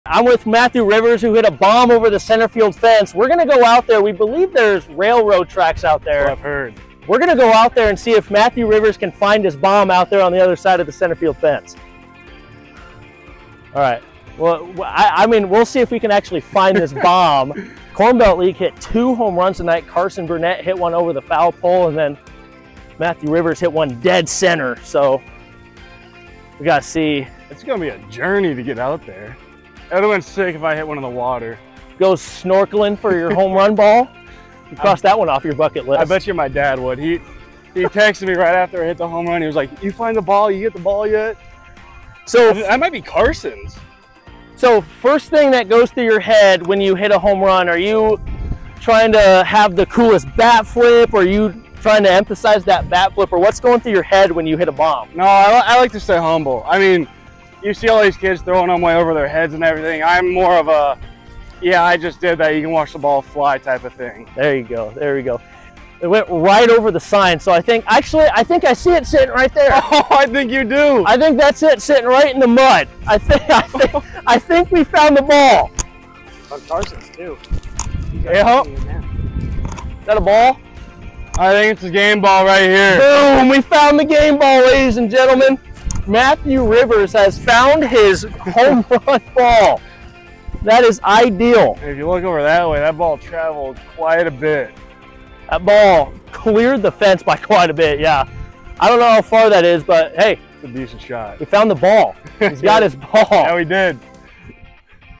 Matthew Rivers' Homerun Adventure
Join 1st baseman Matthew Rivers (DMACC) and broadcaster Ethan McCormick as they hunt for the 420ft homerun Rivers hit against the Dallas-Fort Worth Ravages on Thursday, August 3rd.